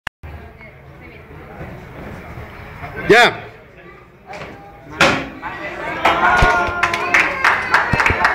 tablas ninos cintas amarillas5
Rompimiento de tablas en el examen del 6 de marzo del 2011